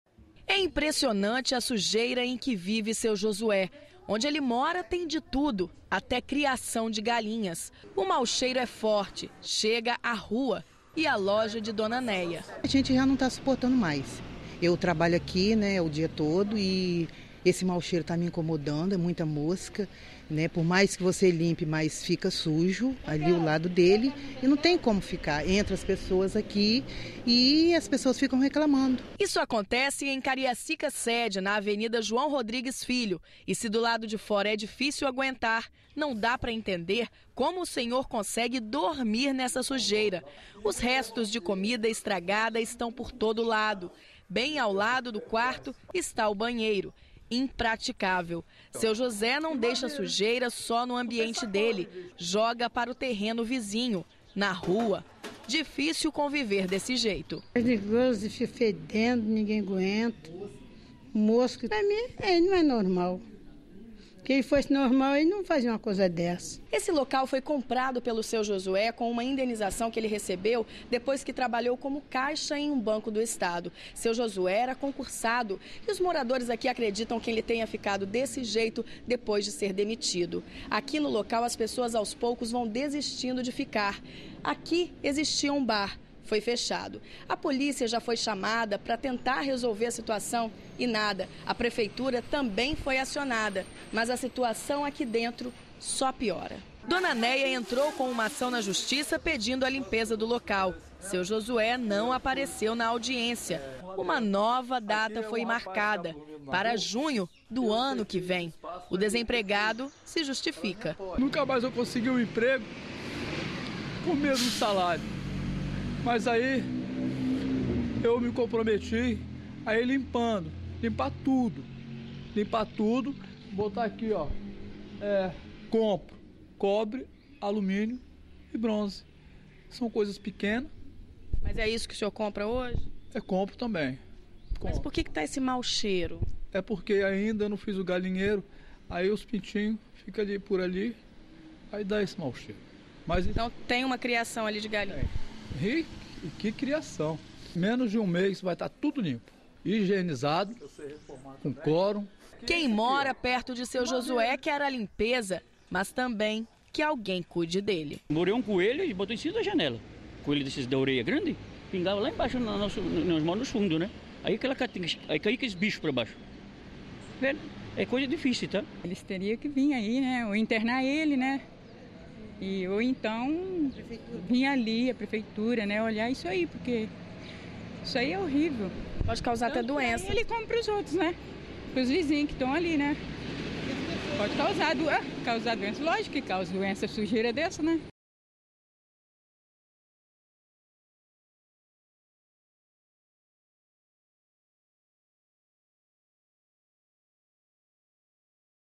Homem acumula lixo em casa e moradores denunciam
Ele revolta a vizinhança e espanta comerciantes com tanto lixo que recolhe das ruas e leva para dentro de casa. Seu Josué fez concurso público, trabalhou 16 anos no banco e foi demitido. Depois disso começou a manter entulhos e animais na residência. Quem mora perto não aguenta mais conviver com os bichos e o mau cheiro.